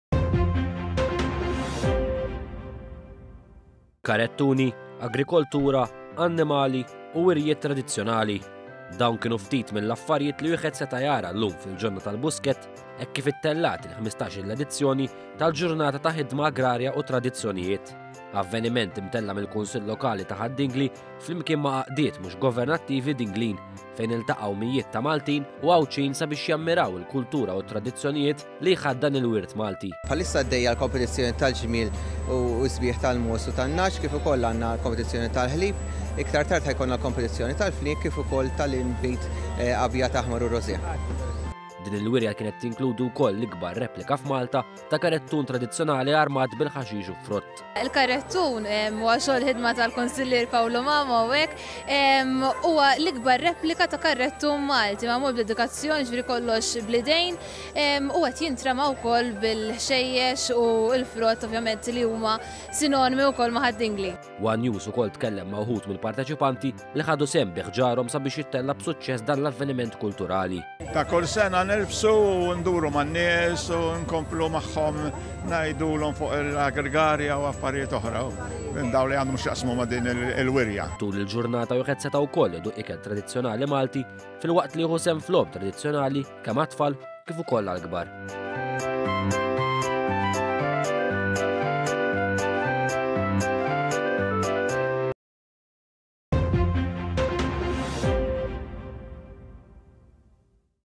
IL-BUSKETT IMUR LURA GĦAŻ-ŻMINIJIET TRADIZZJONALI
ONE.COM.MT • Il-ħarġa tal-Ħadd għal mijiet ta’ Maltin u Għawdxin illum kienet tikkonsisti f’pass lura fiż-żmien hekk kif il-Wirja ta’ Ħidma Agrarja ġiet imtella’ fil-ġonna tal-Buskett.
Wirja li kienet tinkludi diversi wirjiet artiġjanali u demostrazzjonijiet tradizzjonali ta’ ħidma fis-settur agrikolu.